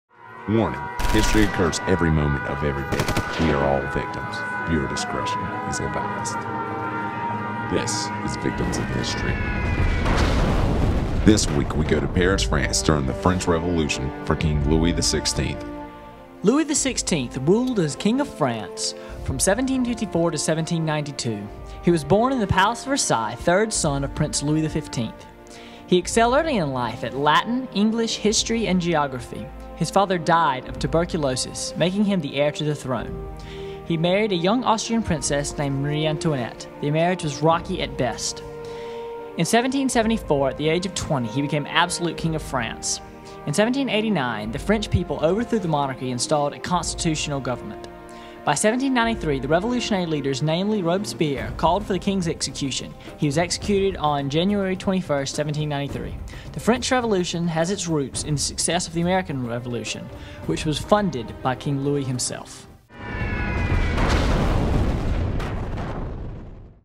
Victims of History - Episode 3 - (Without Voice Synthesizer)
Some people gave complaints that they couldn't understand what was being said in Victims of History so I made a second version of this weeks episode without the voice synthesizer.

From now on I'm going to have two versions of every weeks episode one with the new revamped voice synthesizer and another without it.